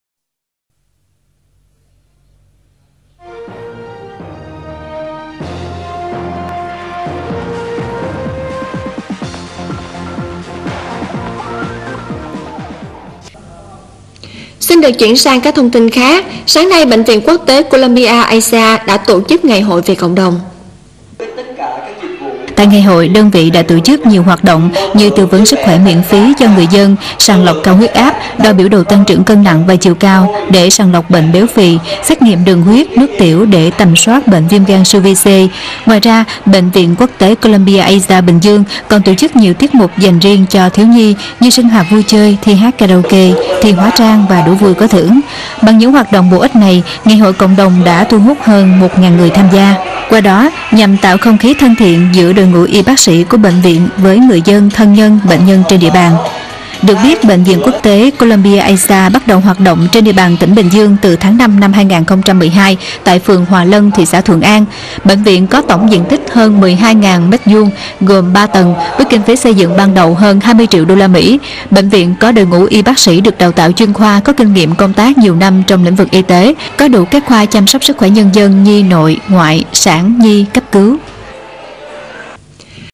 Columbia Asia Binh Duong FIRST Community Day on BTV1
On the 30th of June, our hospital in Binh Duong had their first ever Community Day.